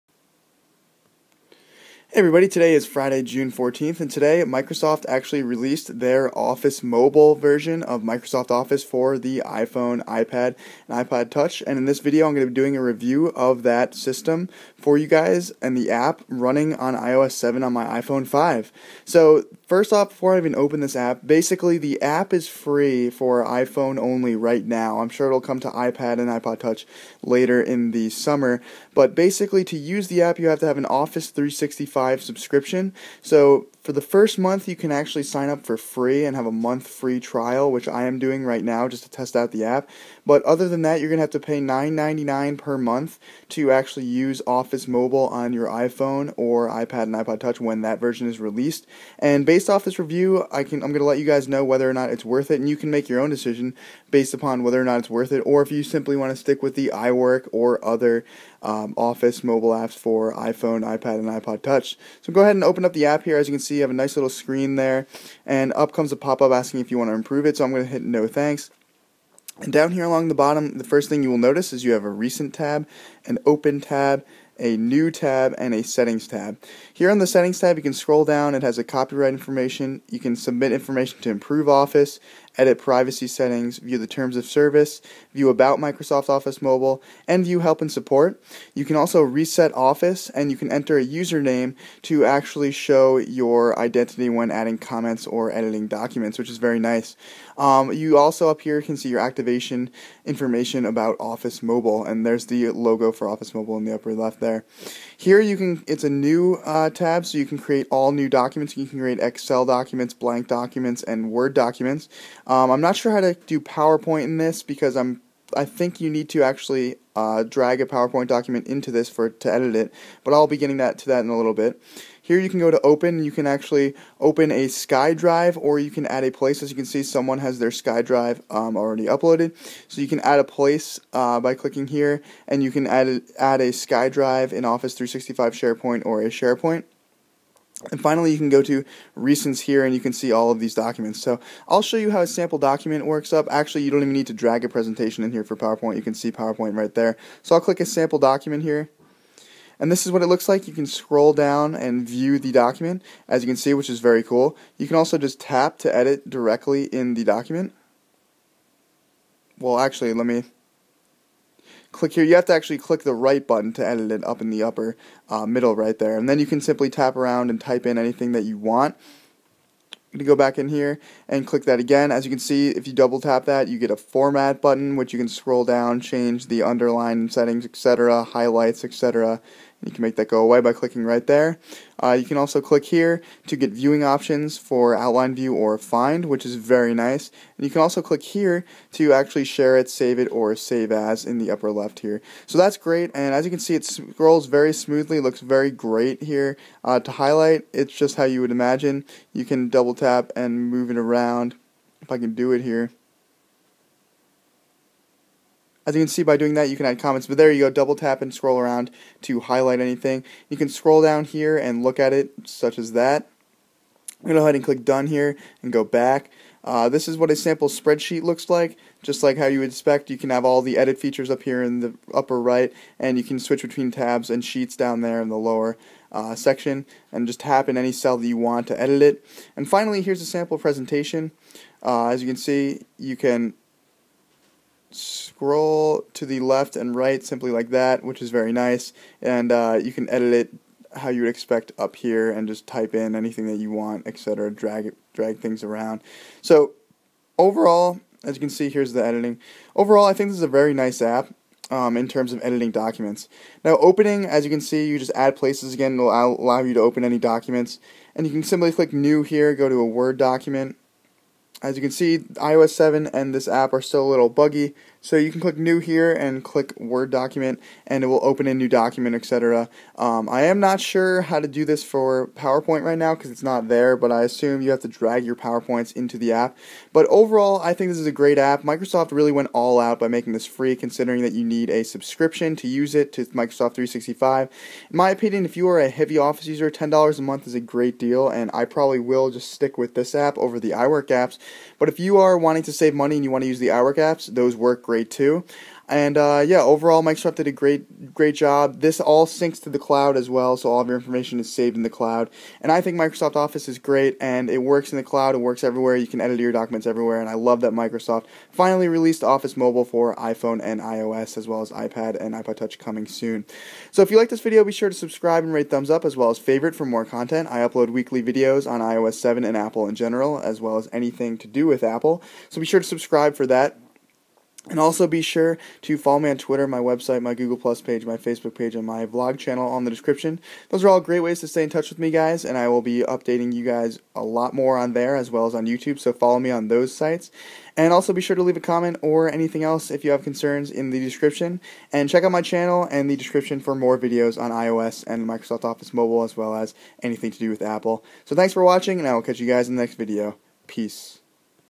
Microsoft Office for iPhone Review: Hands On and First Look!
Microsoft has finally released their Office Mobile App for iOS. In this video, I give you a Hands On Review and First Look of the App! This works on iPhone as of now, but iPad and iPod Touch Versions are coming soon! Features include cloud syncing, document creation and editing of Word, Excel, and Powerpoint docs, and more.